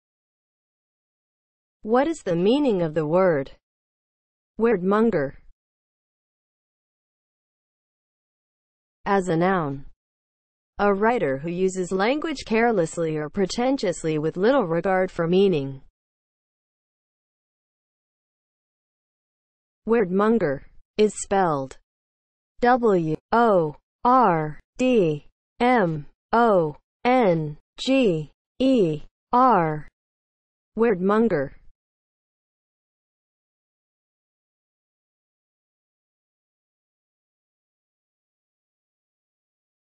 What is the meaning of the word WORDMONGER?
Definition and spelling of the word WORDMONGER.

(00:00) Intro
(00:08) As a noun
(00:18) Spelling

As a noun (00:08):
• a writer who uses language carelessly or pretentiously with little regard for meaning